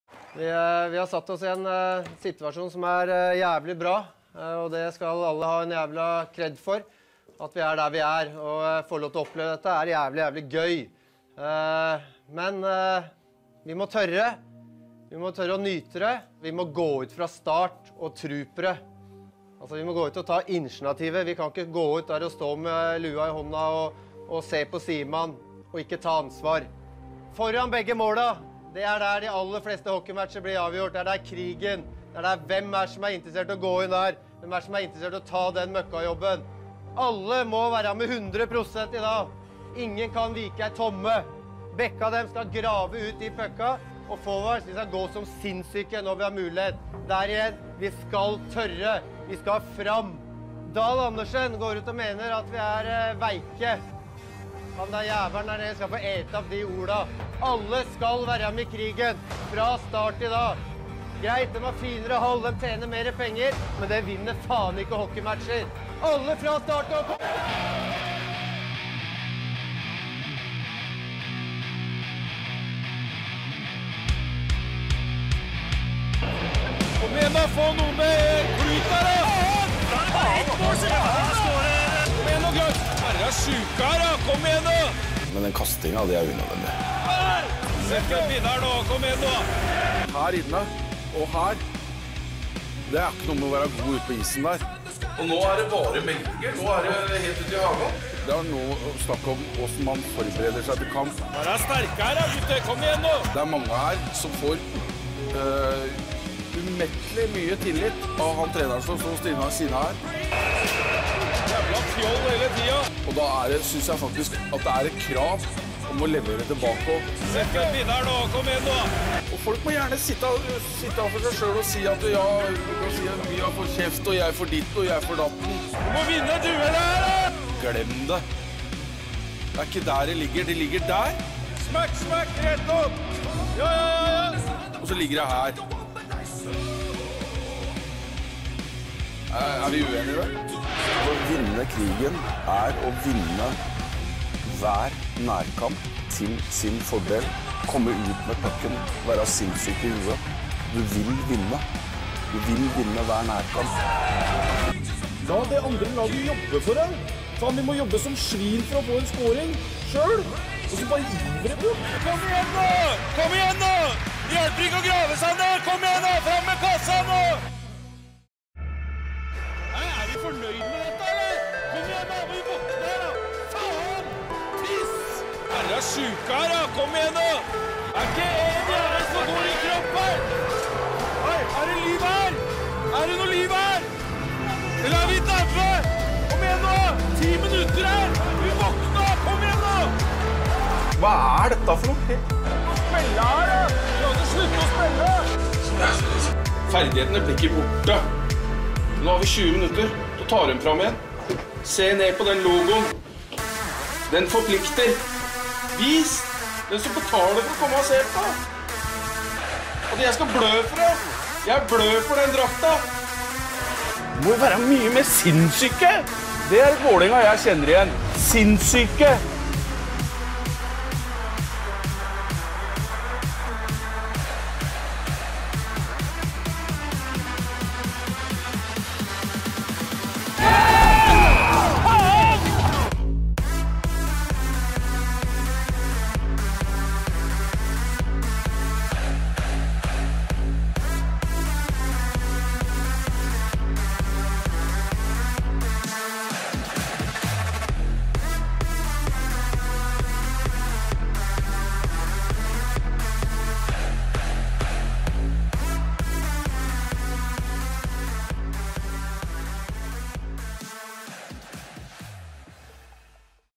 Svenna Pump Up • Feat: Shampo Knutsen
Nyt en ny video av svenna!
Nå Pump up, men denne gangen begynner Sjampo med en punch tale!
Legg alt venn og nyt videoen Max!

Nb: Jeg eier ingen rettigheter for klipp benyttet i denne videoen, og dette produktet er heller ikke produsert for å tjene penger.